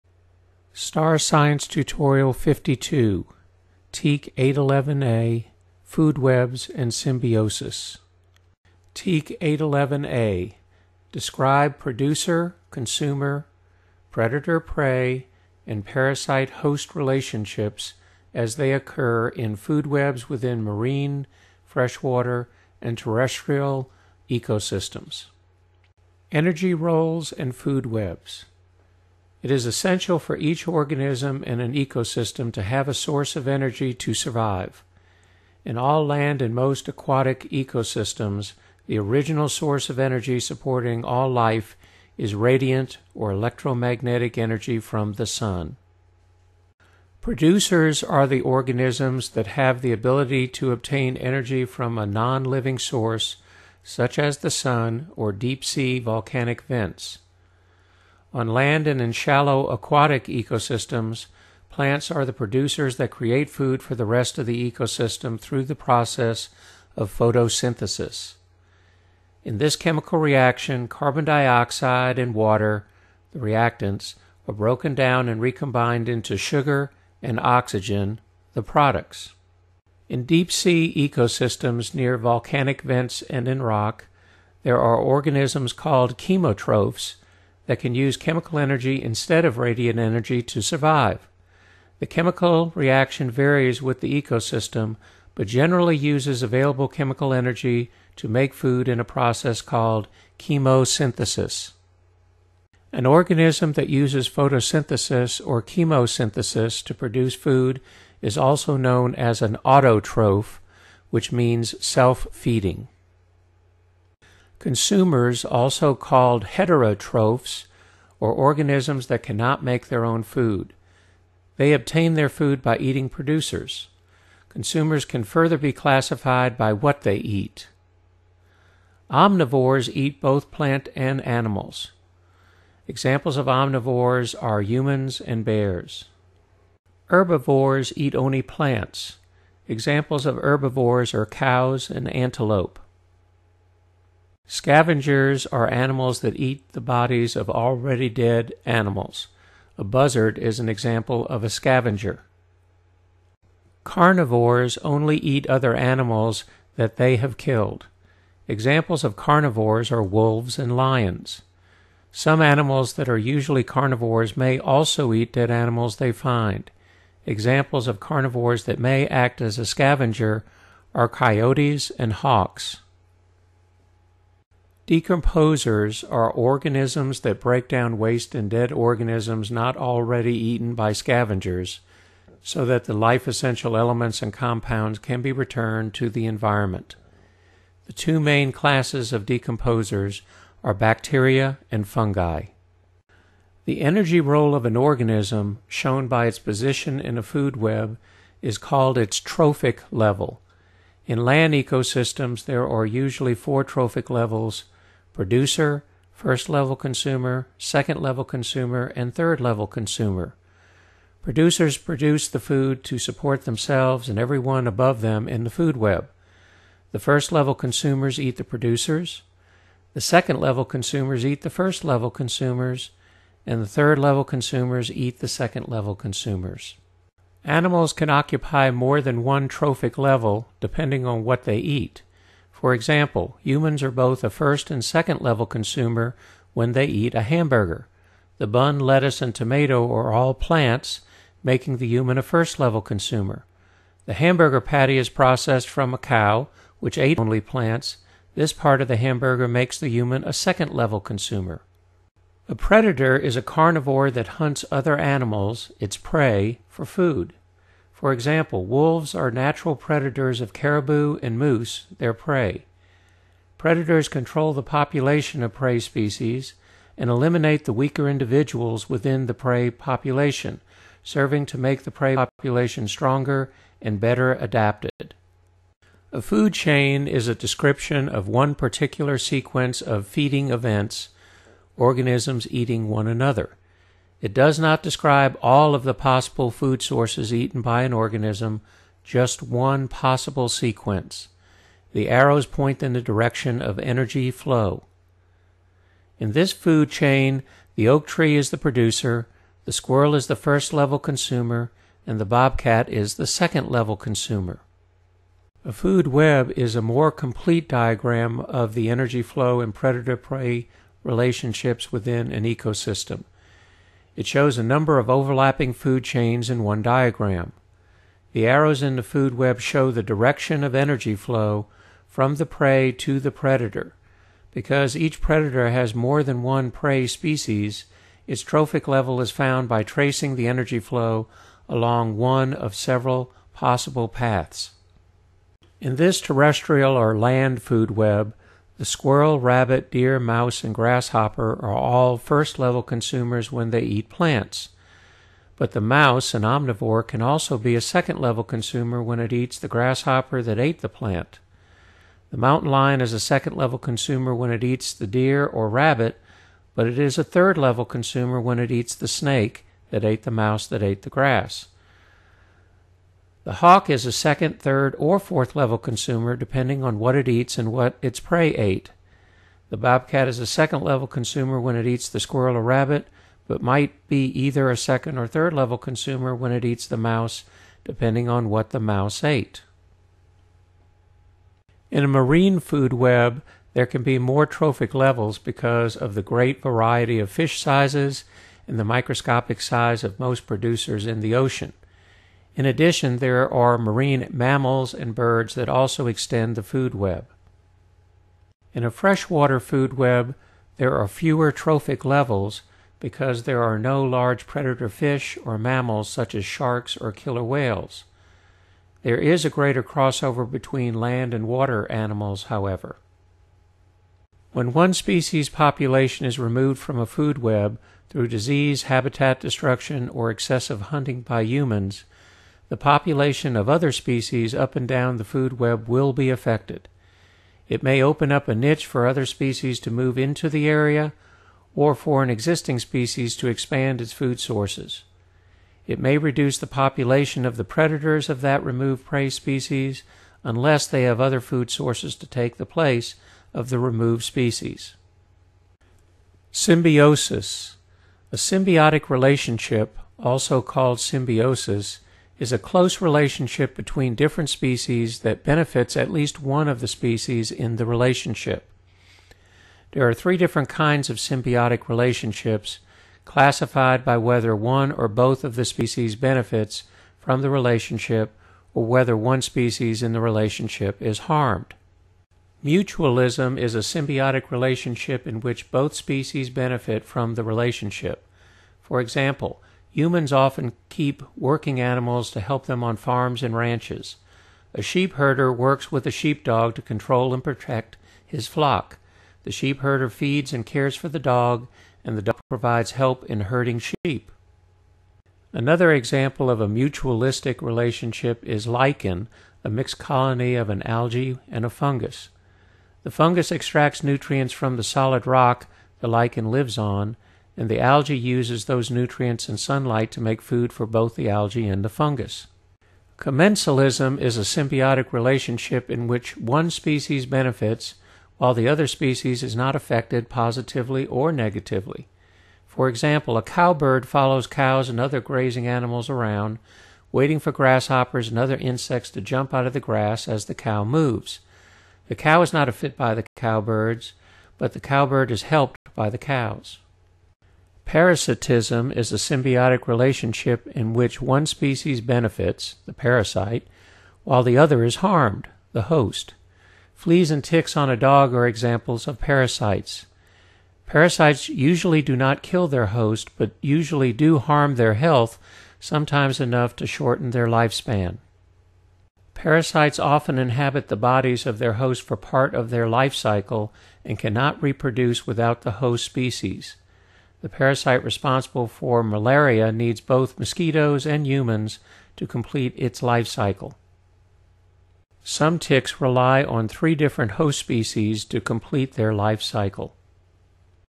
STAAR Science Tutorial 52 Food Webs Symbiosis
This Texas STAAR 8th Grade Science Test tutorial covers predator-prey and symbiotic relationships in ecosystems.